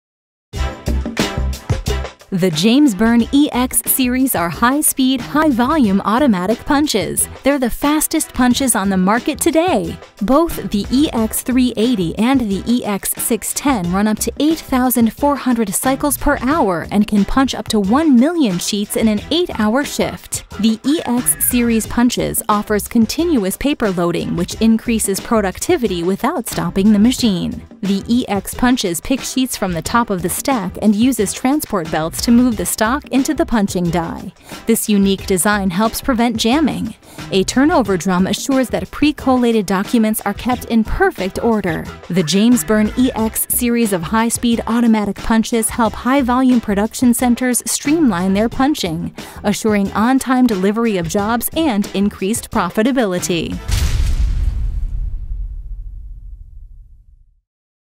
Presenting the James Burn EX Series Automatic Punch Machines [Official Traler]
Learn more about the James Burn EX Series Automatic Punch Machines and many more automatic punches

The James Burn E. X. Series are high speed, high volume, automatic punches. They’re the fastest punches on the market today!!!

Both the EX380 and the EX610 run up to 8,400 cycles per hour and can punch up to one million sheets in an eight hour shift.

The EX Series punches offer continuous paper loading which increases productivity without stopping the machine
The EX punches pick sheets from the top of the stack and uses transport belts to move the stock into the punching die. This unique design helps prevent jamming.

A turnover drum assures that pre-collated documents are kept in perfect order.

The James Burn EX Series of high speed automatic punches help high volume production centers streamline their punching assuring on time delivery of jobs and increased profitability.